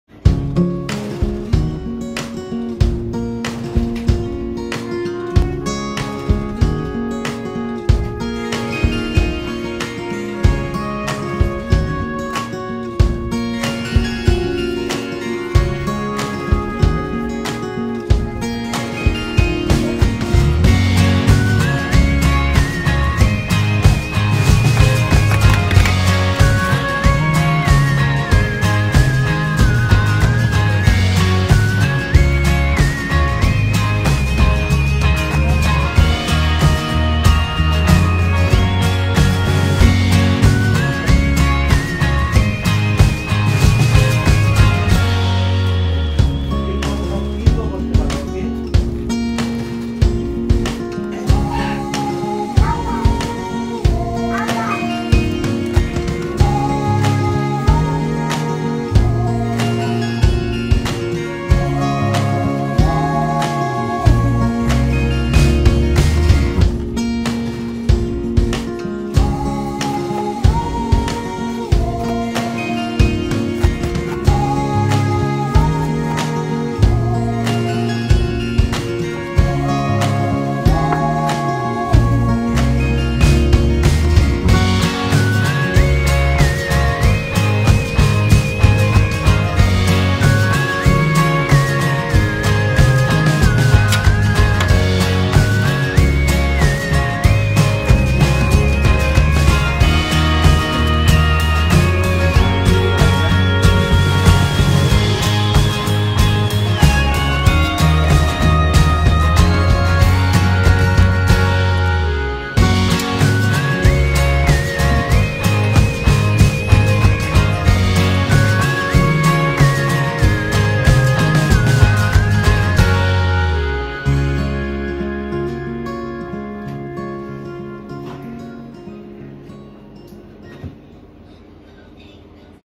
California crispy onion roll / most famous sushi /poke et sushi femme creation
Ingredient
Special rice and sauce
Algue
Saumon
Avcado
Crispy onion
Bamboo sheet
sushi california_crispy_onion_ roll#most_famous_sushi#crispy_onion_sushi#suahi_roll